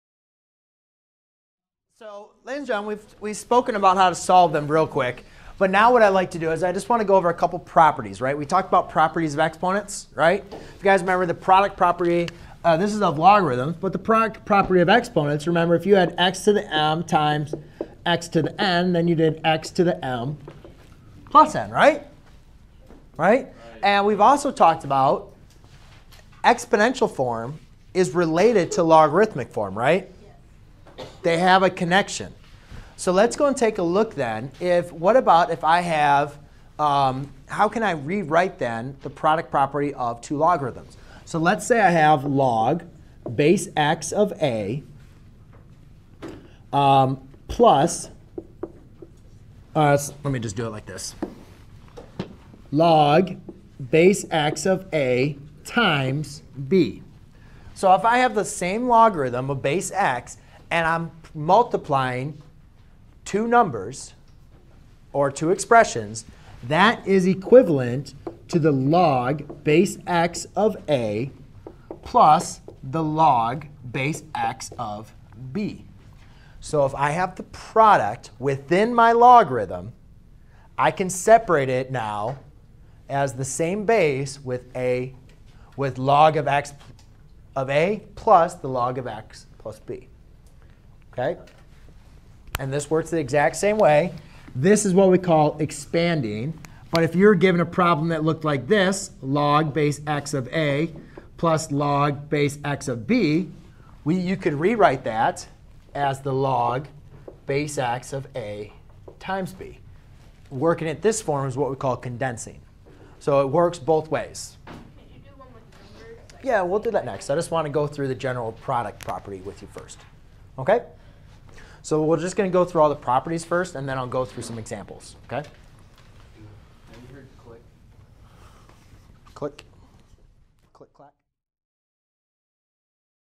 What is the product property of logarithms
👉 Learn about solving logarithmic equations. Logarithmic equations are equations involving logarithms. To solve a logarithmic equation, we first use our knowledge of logarithm laws/properties to express the terms in both sides of the equality sign as single terms. Then, we equate the numbers/expressions in the logarithms and hence solve for the unknown variable. Note that the logarithms must be of the same base.